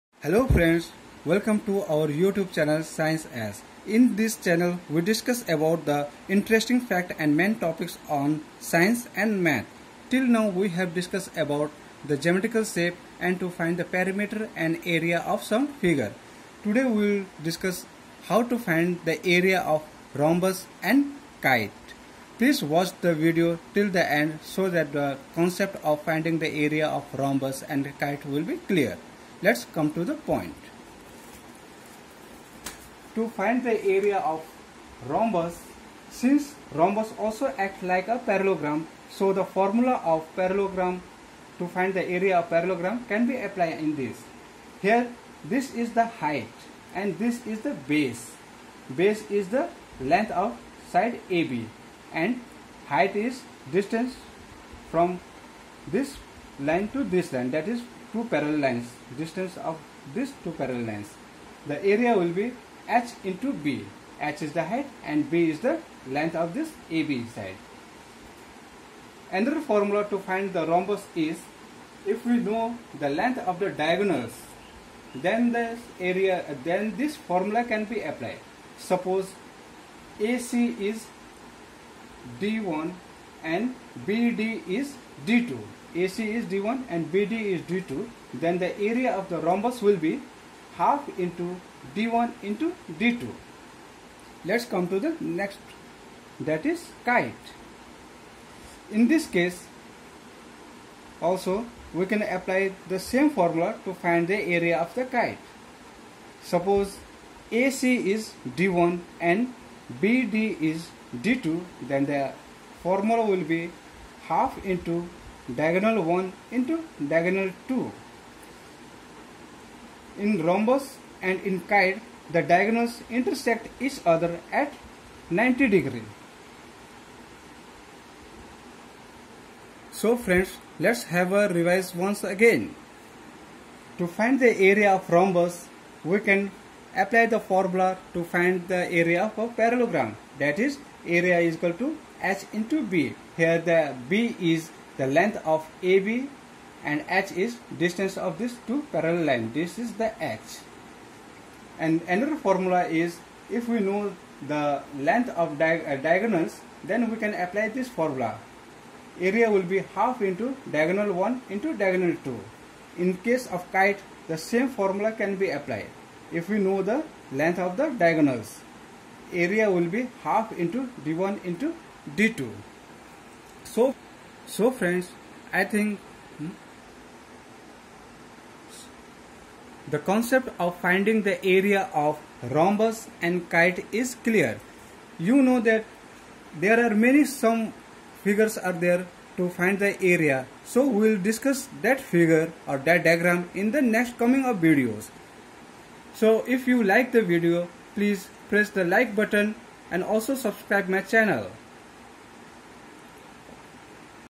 How to find area of Rhombus and Kite.
In this video you will learn how to find area of rhombus and kite.

If you want to see our other video please click here-